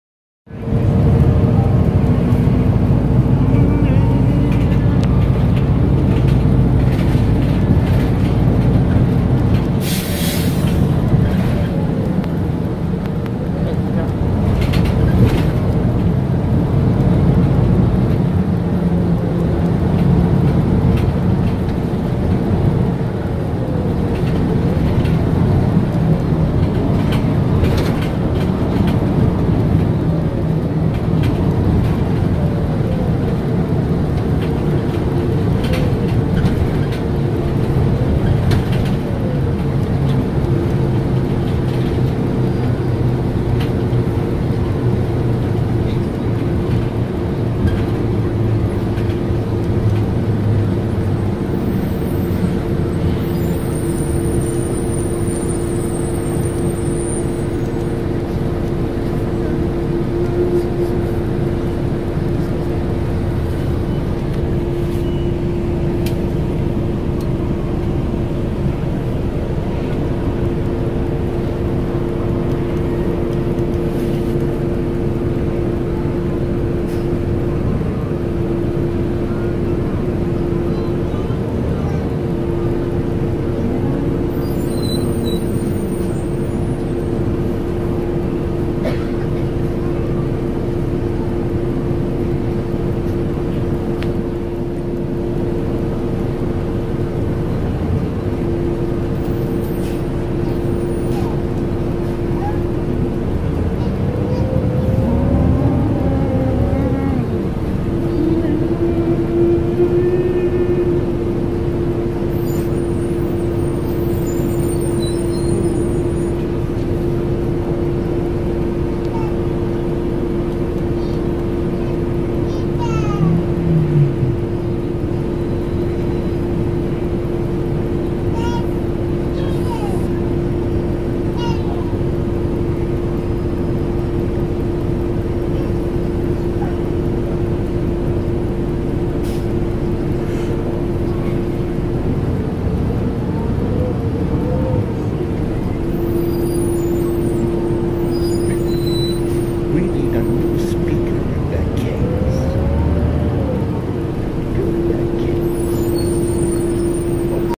NYCTA: On Board 1998-1999 NovaBus RTS-06 Bus #4994 on The B6Limited Bus To Bensonhurst (Retired)
NYCTA: On Board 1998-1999 NovaBus RTS-06 Bus #4994 on The B6Limited Bus To Bensonhurst-Harway Avenue from Albany Avenue and Glenwood Road To Glenwood Road and Nostrand Avenue Part 4

Taken on April 10, 2019